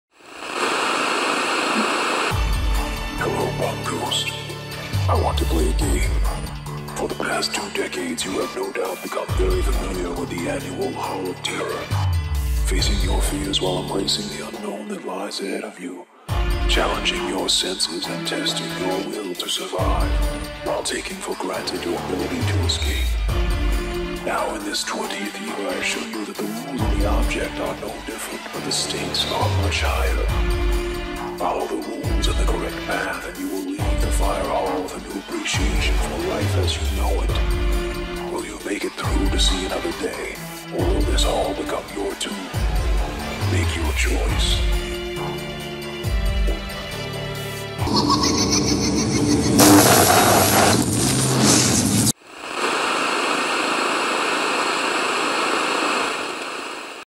Hall of Terror 2021
All halls are about to break loose! Join us if you dare as the Palm Coast Fire Department presents the 20th Anniversary of the Hall of Terror, taking place Friday through Sunday.